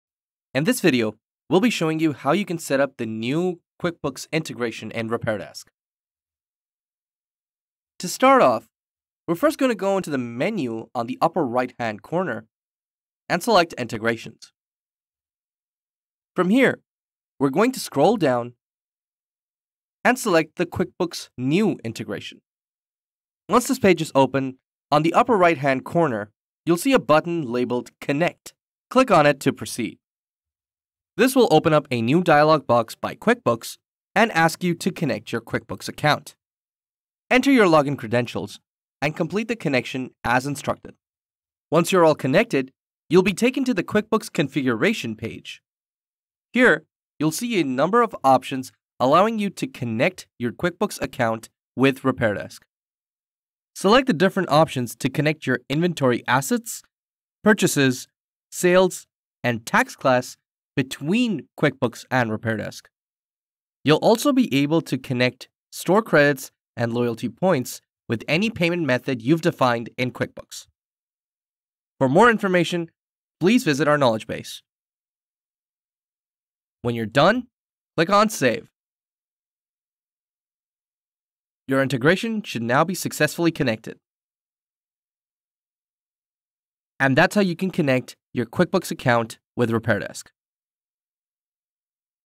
How to Set Up the Quickbooks Integration?
This tutorial focuses on how to set up the Quickbooks integration so you can manage your repair shop's finances well.

We've made this process as simple and easy-to-use as possible while still bringing all the power and versatility of the RepairDesk ecosystem that users will be familiar with.

Learn more about using Quickbooks with RepairDesk on the RepairDesk Help Center